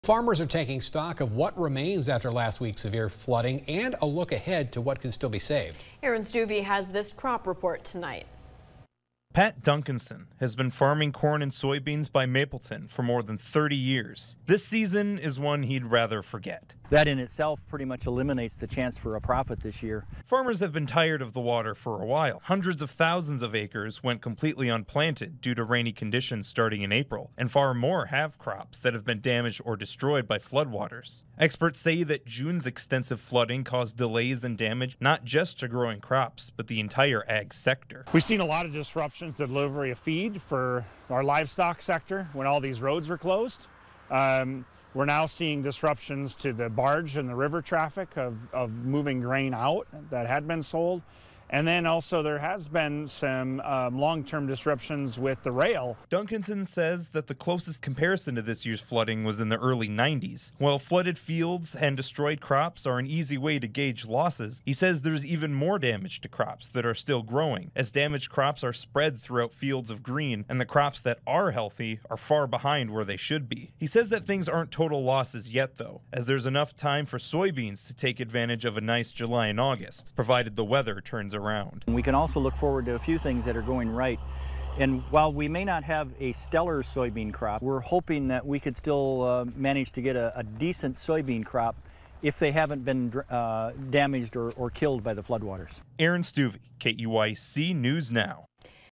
Area farmers take inventory following floods
Farmers are taking inventory of what remains after last week's severe flooding, and looking ahead to what can still be saved.